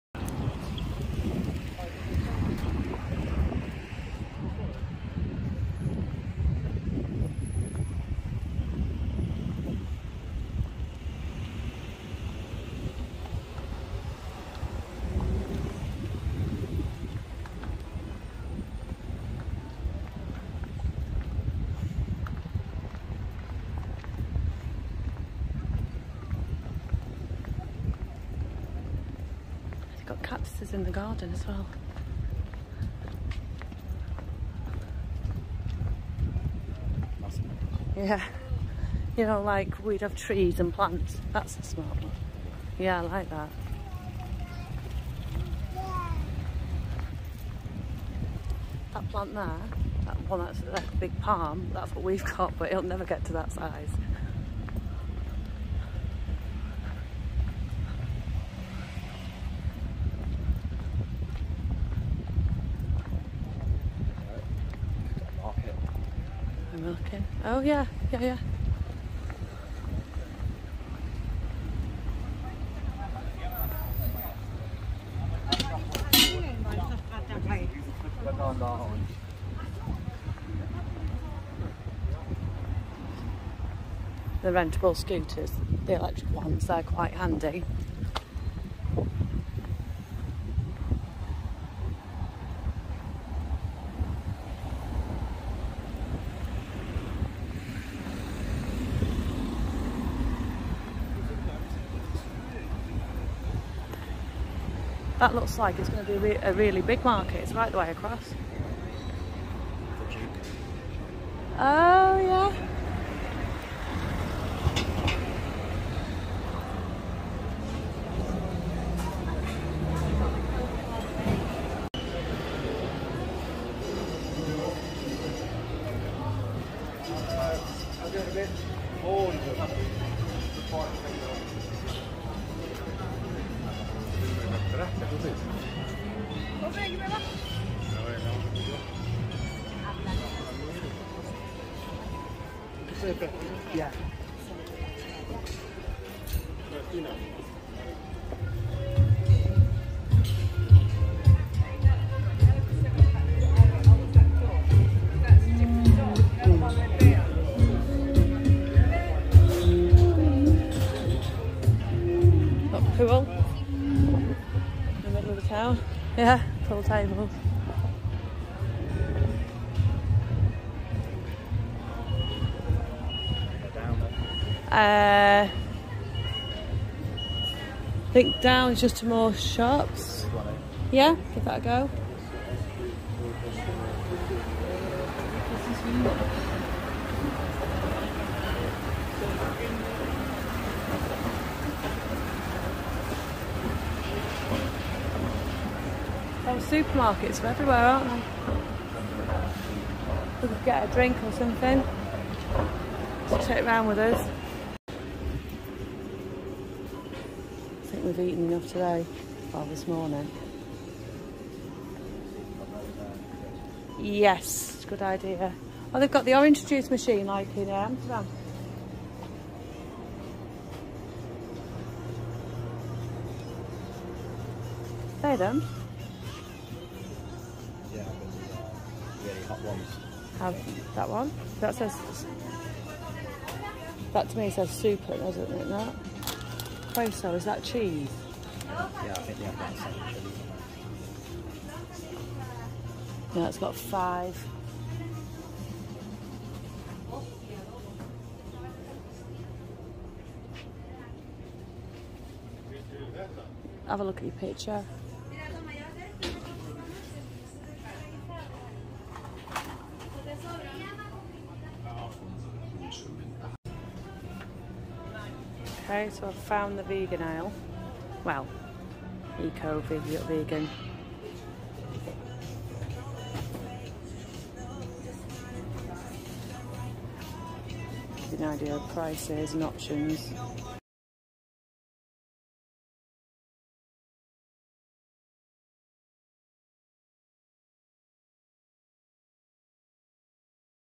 Tenerife Designer shops, restaurants, supermarket vegan food hunt - Duke shopping centre Adeje
A walk to the Duke Shopping Centre full of designer clothes, restaurants. lovely and clean calm atmosphere